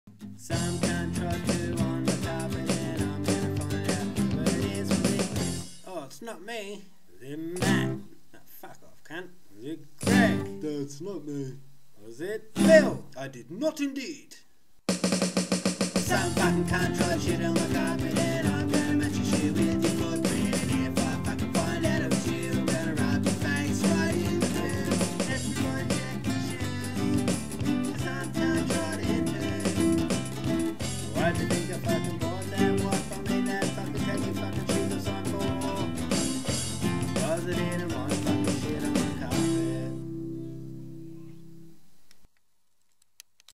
Arse Eyes - The Mystery Shitter
Some cunt trod poo on my carpet and I'm gonna find out who it is!